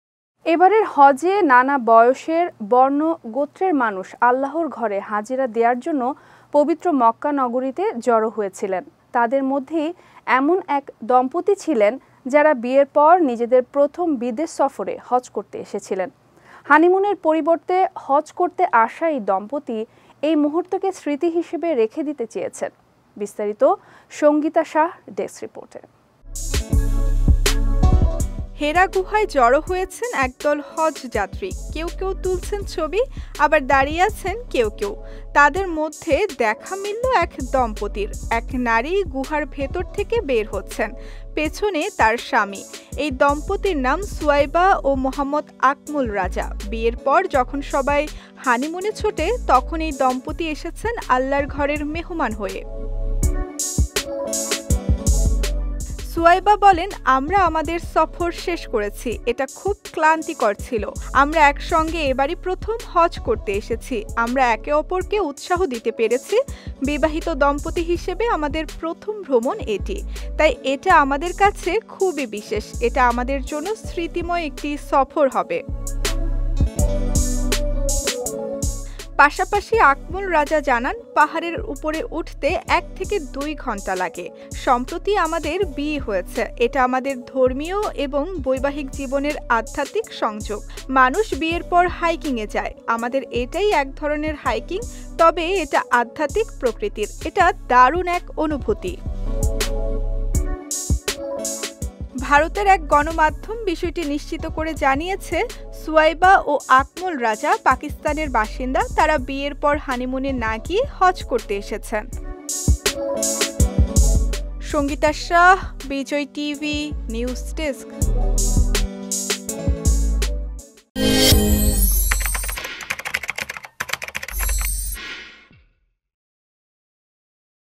বিয়ের পর হানিমুনে না গিয়ে হজে গেলেন দম্পতি! Bijoy TV
এবারের হজে নানা বয়সের, বর্ণ-গোত্রের মানুষ আল্লাহর ঘরে হাজিরা দেয়ার জন্য পবিত্র মক্কা নগরীতে জড়ো হয়েছিলেন। তাদের মধ্যেই এমন এক দম্পতি ছিলেন, যারা বিয়ের পর নিজেদের প্রথম বিদেশ সফরে হজ করতে এসেছিলেন। হানিমুনের পরিবর্তে হজ করতে আসা এই দম্পতি, এই মুহূর্তকে স্মৃতি হিসেবে রেখে দিতে চেয়েছেন।
হেরা গুহায় জড়ো হয়েছেন একদল হজযাত্রী। কেউ কেউ তুলছেন ছবি। আবার দাঁড়িয়ে আছেন কেউ কেউ। তাদের মধ্যে দেখা মিলল এক দম্পতির। এক নারী গুহার ভেতর থেকে বের হচ্ছেন। পেছনে তার স্বামী। এই দম্পতির নাম সুআইবা ও মোহাম্মদ আকমল রাজা। বিয়ের পর যখন সবাই হানিমুনে ছোটে, তখন এই দম্পতি এসেছেন আল্লাহর ঘরের মেহমান হয়ে।